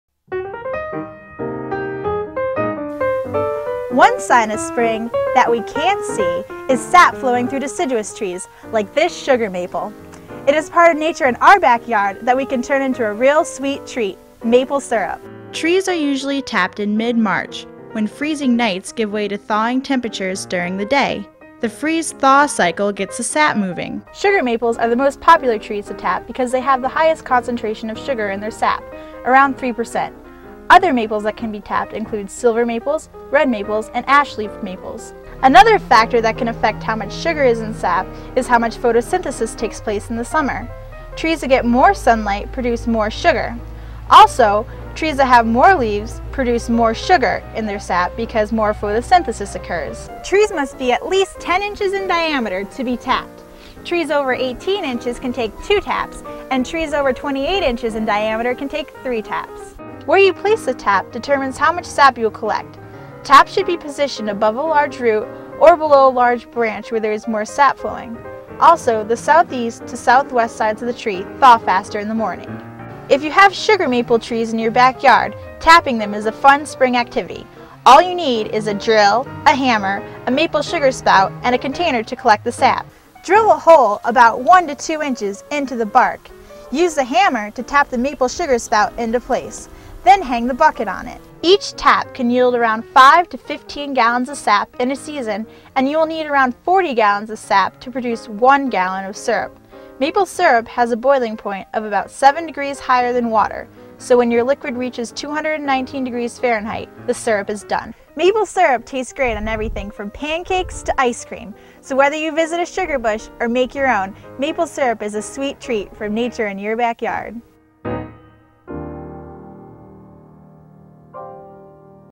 Maple Syrup: Why and How?
A naturalist from the SUNY College of Environmental Forestry discusses Maple Syrup.